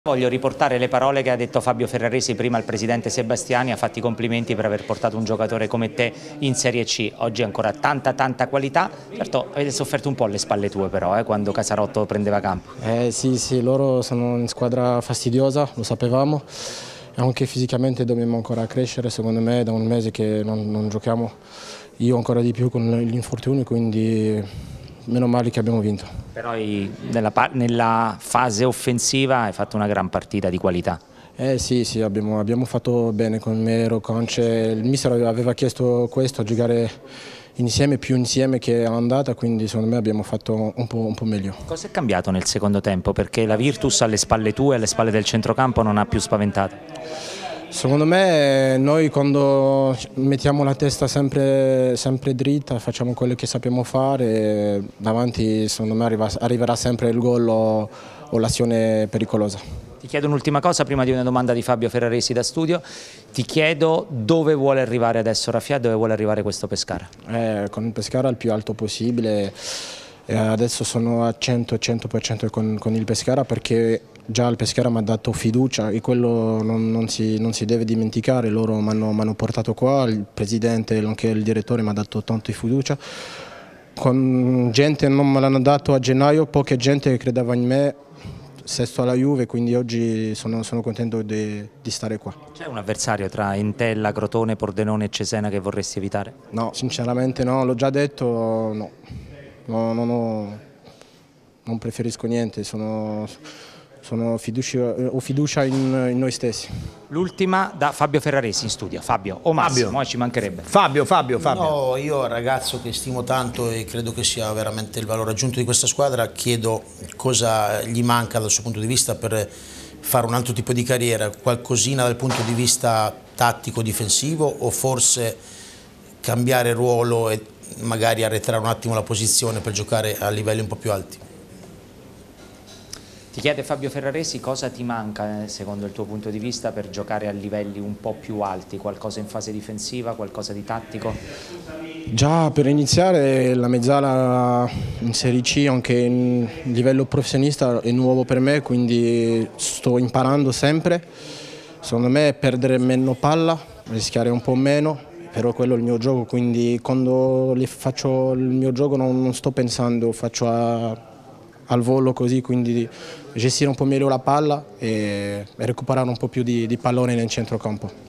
Pescara - Virtus Verona 3-1 Rafia: "Pescara mi ha dato fiducia, voglio ripagare"
Serie C - Playoff - FASE NAZIONALE - OTTAVI 22|05|2023